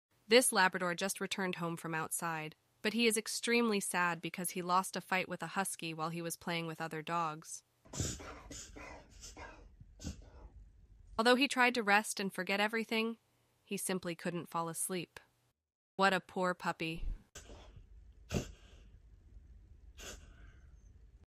Sad Labrador lost a fight with a husky
This Labrador just returned home from outside, but he is extremely sad because he lost a fight with a husky while he was playing with other dogs. Although he tried to rest and forget everything, he simply couldn't fall asleep. What a poor puppy.